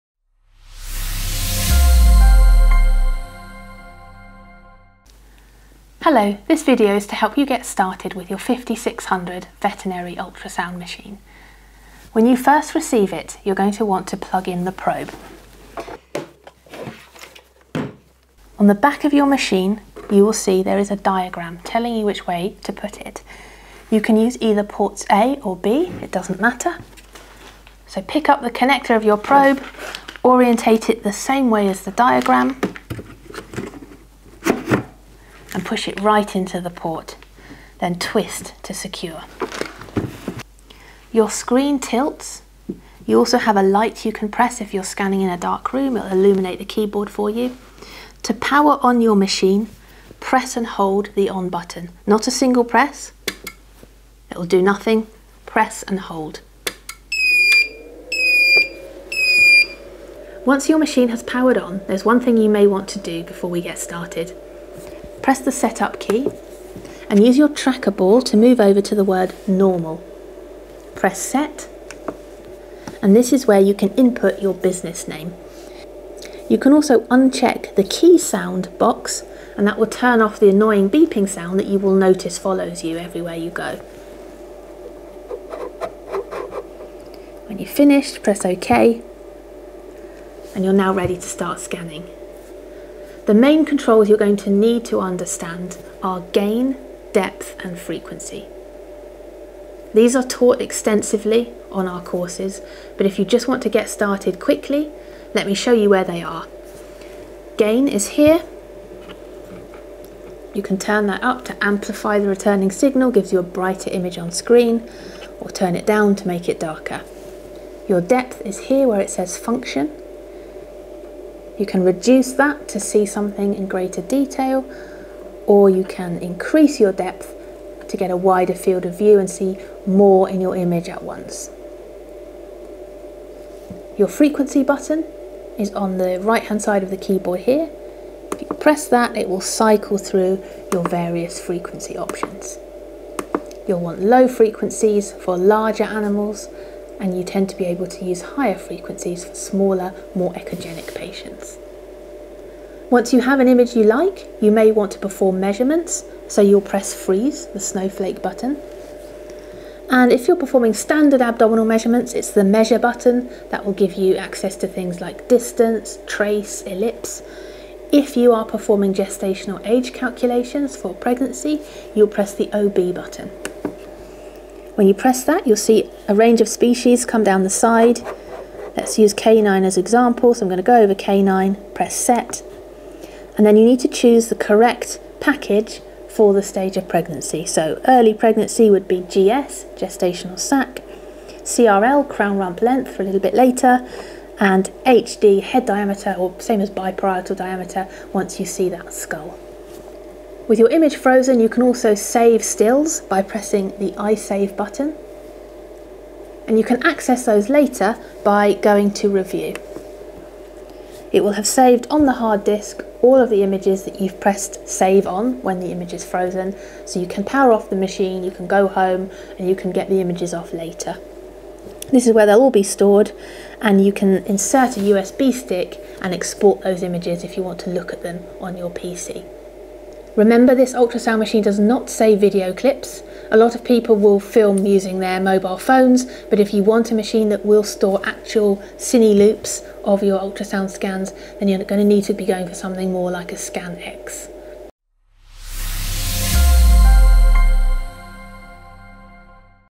Getting started with the KX5600v veterinary ultrasound machine
Here's how to get started with the Elite 5600 / KX5600v veterinary ultrasound machine. If you are in the UK, Europe or Australia, you can see this machine

If you are in North America, you can see this machine

If you would like to save videos, view the ScanX here (UK website) (North American website).